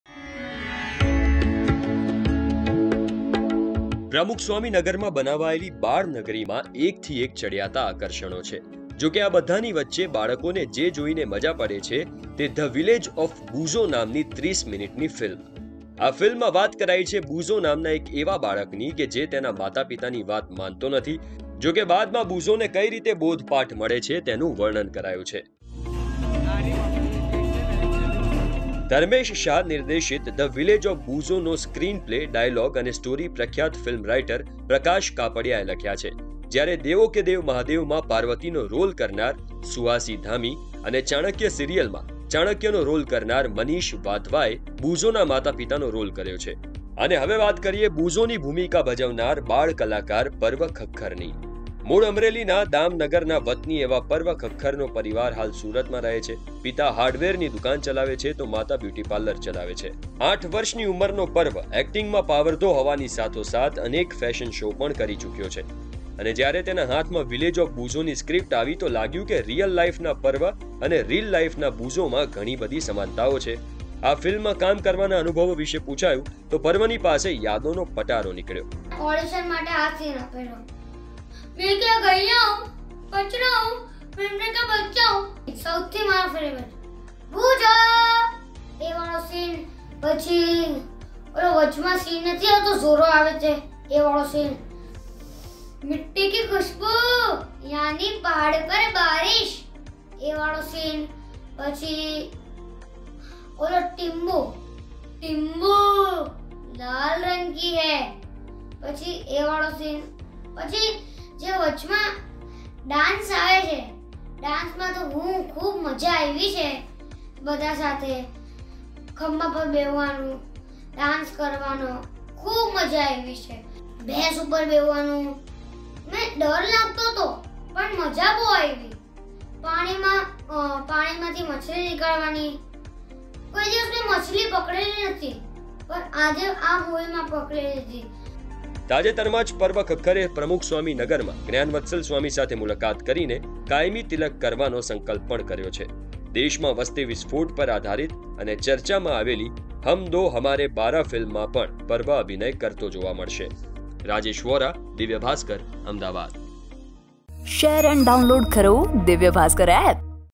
એક્સક્લૂસિવ : ગુજરાતનાં બાળકોને ઘેલું લગાડનાર ‘બુઝો’
એક્સક્લૂસિવ : ગુજરાતનાં બાળકોને ઘેલું લગાડનાર ‘બુઝો’, હિંદી ફિલ્મમાં કામ કરનારા પર્વનો પરિવાર અમરેલીનો

ફિલ્મમાં કોણ બન્યું છે ‘બુઝો’નાં માતા-પિતા?
ધર્મેશ શાહ નિર્દેશિત ‘ધ વિલેજ ઓફ બુઝો’નો સ્ક્રીન પ્લે, ડાયલોગ અને સ્ટોરી પ્રખ્યાત ફિલ્મ-રાઇટર પ્રકાશ કાપડિયા(દેવદાસ, બ્લેક,બાજીરાવ મસ્તાની, પદ્માવત, તાન્હાજી: ધ અનસંગ વોરિયર, ગંગુબાઈ કાઠિયાવાડી)એ લખ્યા છે. ‘ધ વિલેજ ઓફ બુઝો’માં ‘દેવો કે દેવ મહાદેવ’માં પાર્વતીનો રોલ કરનાર સુહાસી ધામી અને ચાણક્ય સિરિયલમાં ચાણક્યનો રોલ કરનાર મનીષ વાધવાએ બુઝોનાં માતા-પિતાનો રોલ કર્યો છે.

ફિલ્મમાં બુઝો તેમનાં માતા-પિતાને પૂછે છે કે‘કોઈ ભી ચીઝ આસમાન મેં ઉછલતી હૈ વો નીચે હી ગીર જાતી હૈ પર આસમાન ક્યું નીચે નહીં ગીરતા’, ‘હમ પૈર સે ચલતે હૈ, પરીંદે પંખ સે ઉડતે હૈ, બાદલો કે પાસ ના તો પૈર હૈ ના પંખ તો વો કૈસે ચલતે હૈ’જો કે બુઝોના આ સવાલોના જવાબ તેના માતા-પિતા પાસે હોતા નથી. રિલની જેમ રિયલ લાઇફમાં પણ બુઝો એટલે કે પર્વ ખખ્ખર આવો જ છે. દિવ્ય ભાસ્કરે ‘ધ વિલેજ ઓફ બુઝો’માં બુઝોનું પાત્ર ભજવનાર 8 વર્ષના પર્વ ખખ્ખર, તેમના માતા રિદ્ધી ખખ્ખર, ફિલ્મના રાઇટર પ્રકાશ કાપડિયા, ફિલ્મમાં બુઝોના માતા-પિતાનો રોલ કરનાર મનીષ વાધવા અને સુહાસી ધામી સાથે એક્સક્લુઝિવ વાતચીત કરી હતી